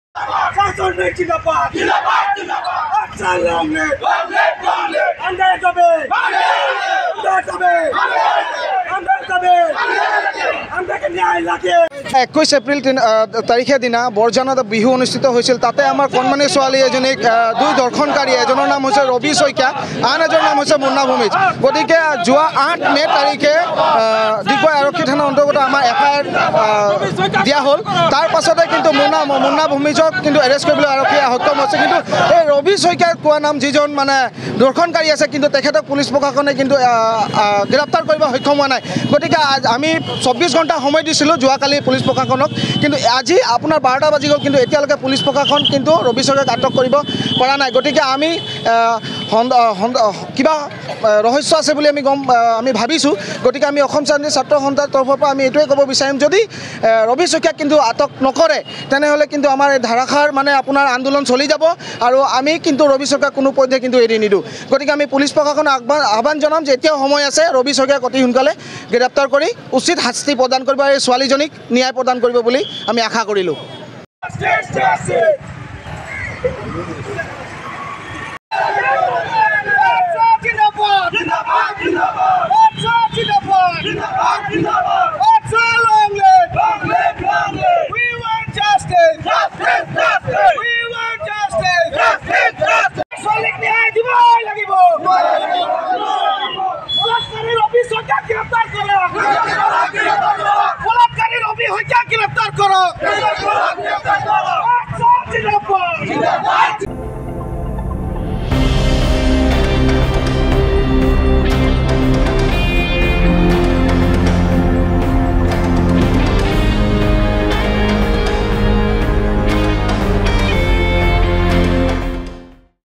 অসম চাহ জনগোষ্ঠী ছাত্ৰ সন্থা ডিগবৈ সমিতিৰ ৰাষ্ট্ৰীয় ঘাইপথ অৱৰোধ কাৰ্যসূচী।
today assam 2024
today nalbari 2024
nalbarir khabar
asomar khabar
asomiya news